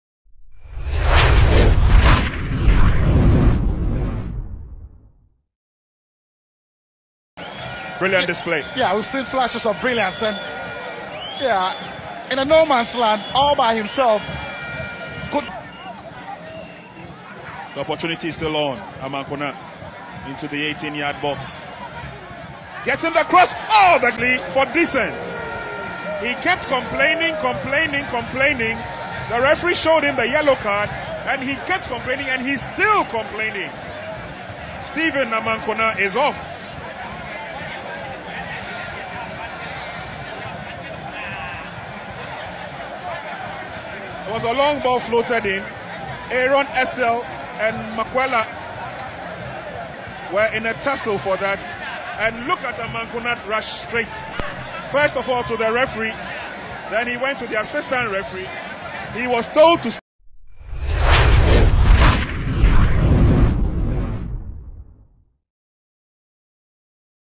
Watch why Kotoko's Stephen Amankona was sent off against Bechem United
Watch why Asante Kotoko's Stephen Amankona was sent off against Bechem United in their 2-0 defeat